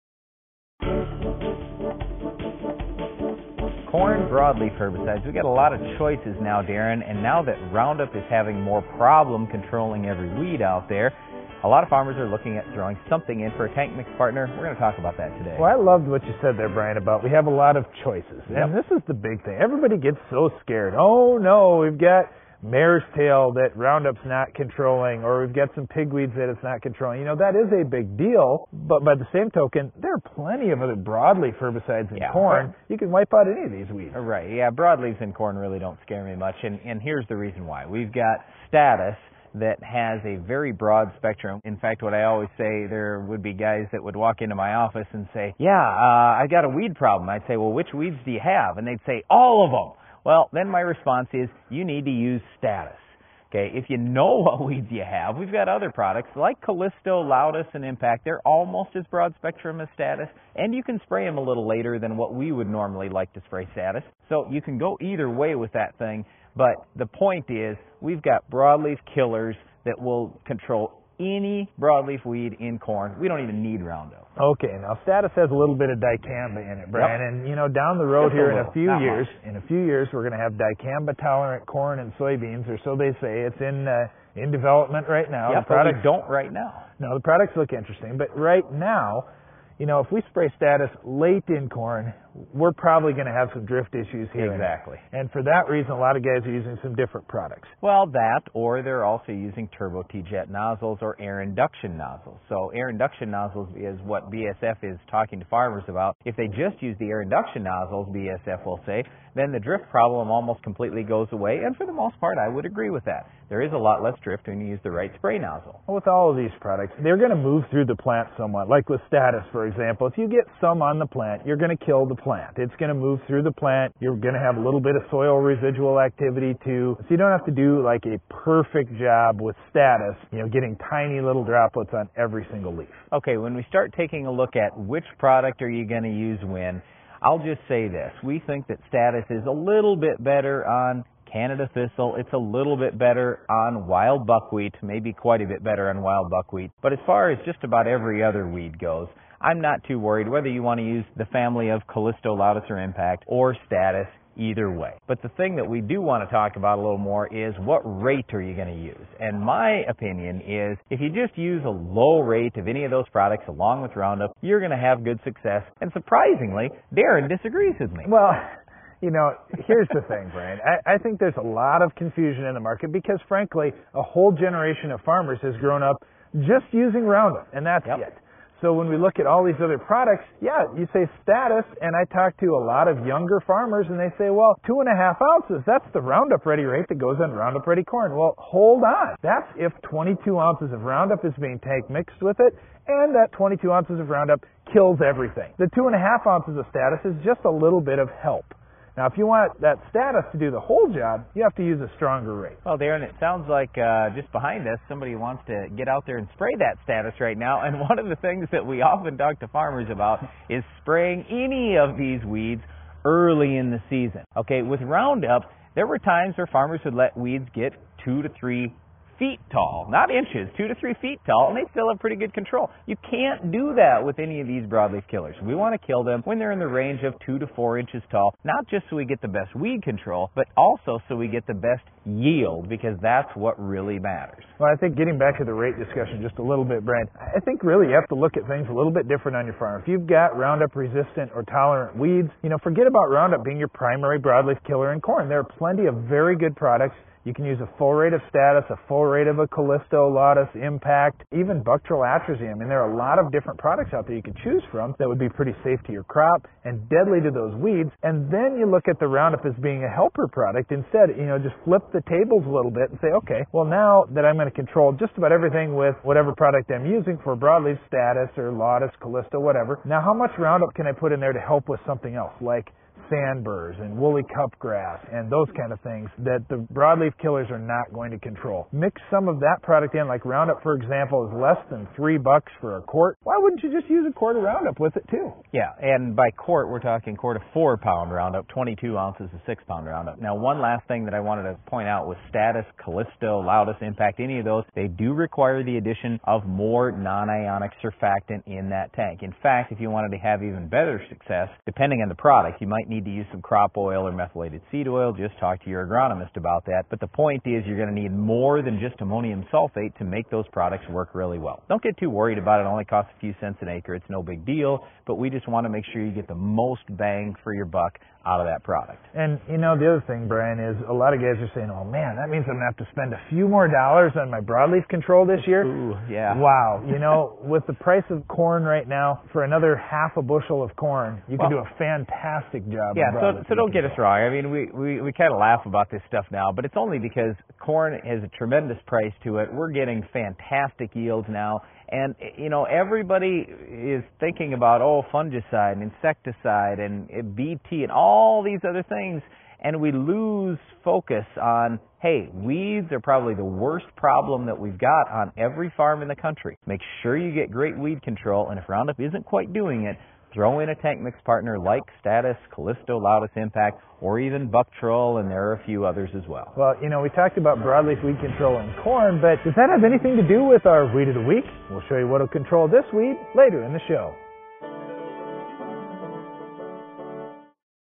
Corn Broadleaf Herbicides #682 (Air Date 5/1/11)
Brian & Darren discuss broadleaf herbicides for corn.